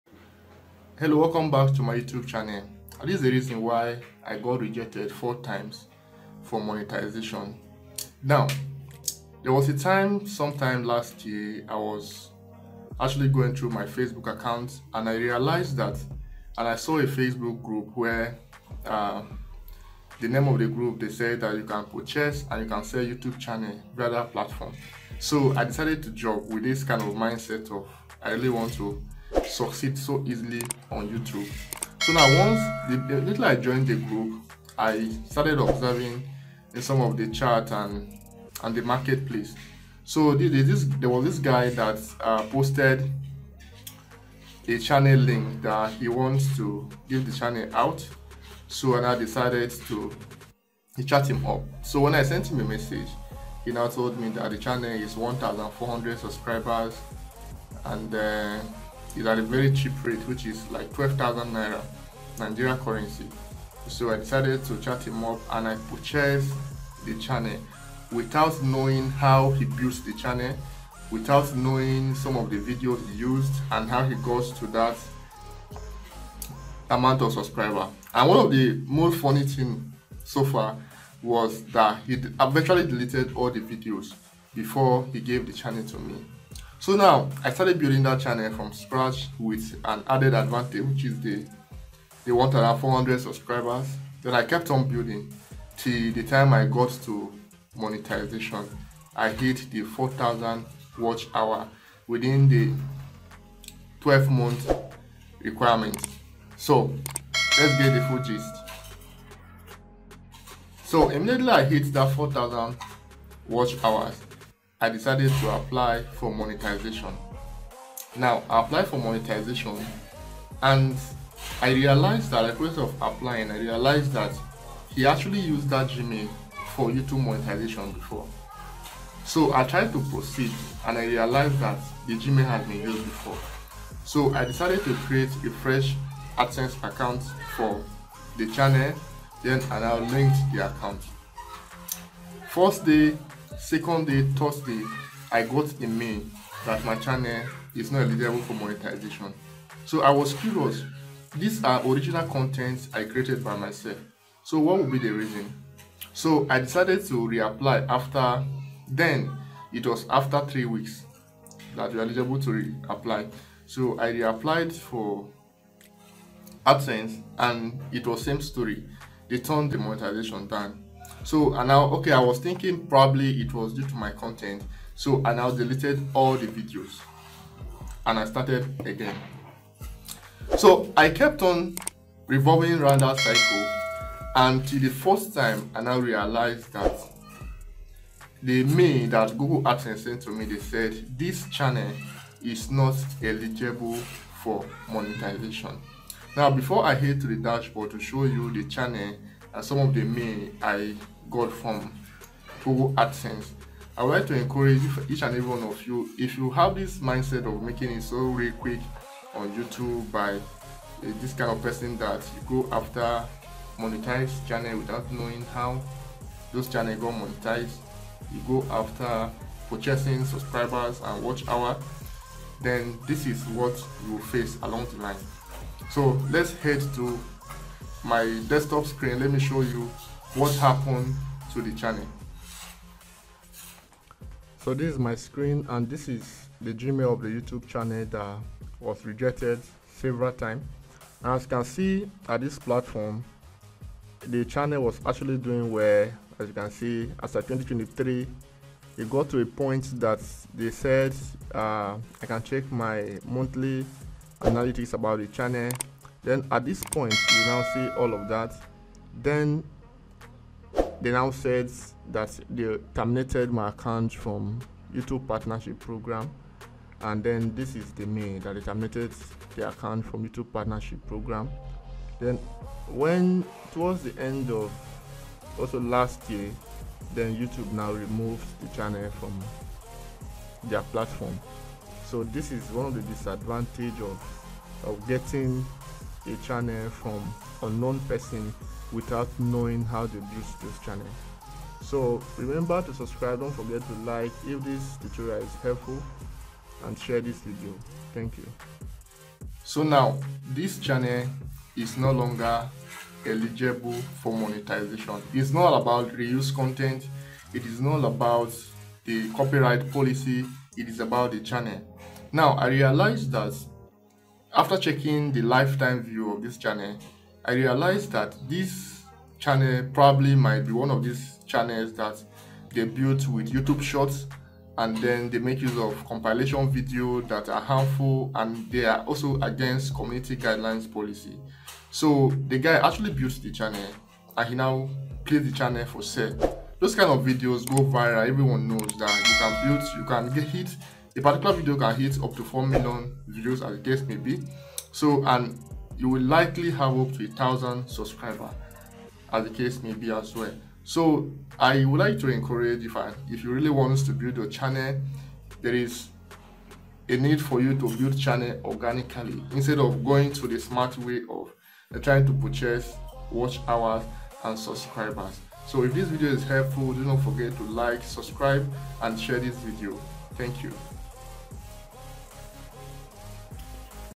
I got rejected for monetization four times on YouTube. Here is why?
this video explain why my channel got rejected four time for monetization